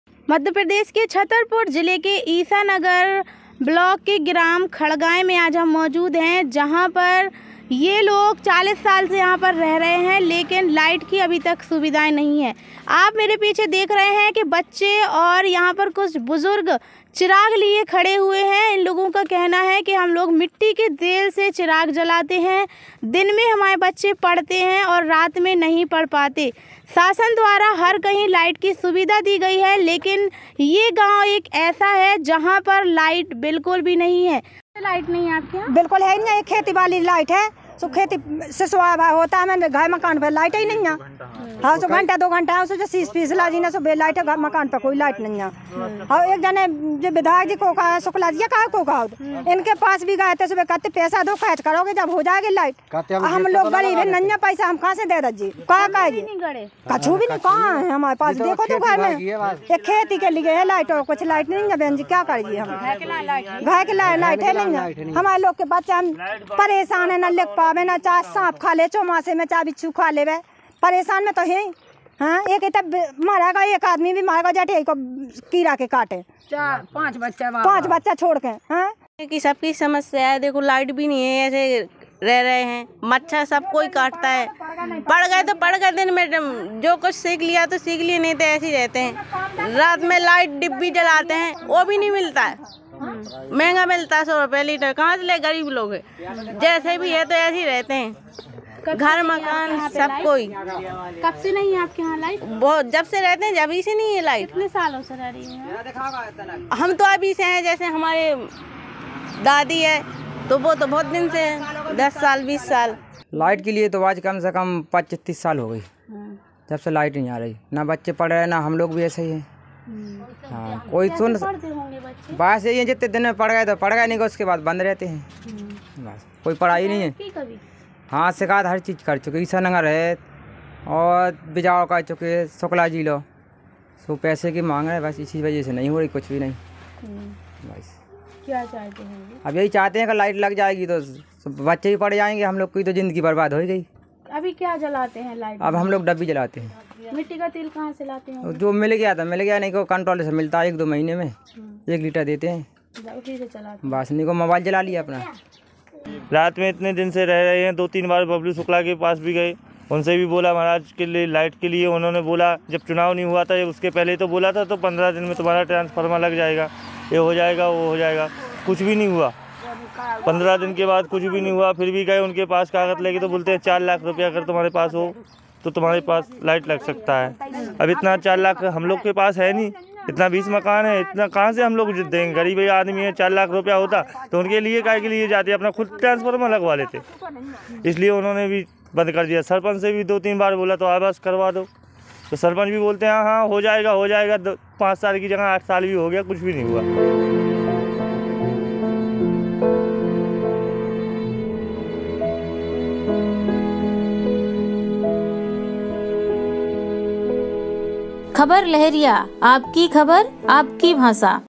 गर्मी में भी बिजली की सुविधा से वंचित गाँव| KhabarLahariya
छतरपुर : बिजली की समस्या से इस समय कई जिलें प्रभावित हैं लेकिन ईसानगर ब्लॉक, ग्राम गौशाला गांव में बिजली न होने की समस्या पिछले 5 सालों से हैं। गाँव की आबादी 60 से 70 लोगों की है। यहां लोग अभी भी मिट्टी का तेल उजाले के लिए और बच्चे पढ़ाई के लिए इस्तेमाल करते हैं। तेल महंगा होने की वजह से अब वह मिट्टी के तेल का इस्तेमाल भी सिर्फ 1 घंटे ही उजाले के लिए करते हैं। वहीं उनका बाकी का समय अँधेरे में बीतता है।

बच्चों ने बताया कि बिजली नहीं रहती इसलिए वह पढ़ाई दिन में ही करते हैं। वहीं गाँव वाले कहते हैं कि बिजली न होने से व अँधेरे की वजह से दुर्घटनाएं हो जाती हैं। कोई शिकायत करता है तो आरोपी कहते हैं कि अँधेरा होने की वजह से ऐसा हुआ है। इस मामले में सुनवाई भी नहीं होती।

बिजली विभाग के अधिकारी एस.एस. गुप्ता से खबर लहरिया ने बात की। उनका कहना था कि घर कम है इसलिए लाइट लगवाना मुश्किल है। अभी उनके पास लाइट लगवाने के लिए बजट भी नहीं आया है। वैसे भी लाइट नहीं आ रही है। अगर लोग साथ दें तो वह लाइट लगवाने की सोच सकते हैं। आगे कहा, अगर जनसंख्या ज़्यादा होती तो ज़रूर से लाइट लग जाती। फिर भी वह कोशिश करेंगे कि लोगों को सुविधा मिल पाये।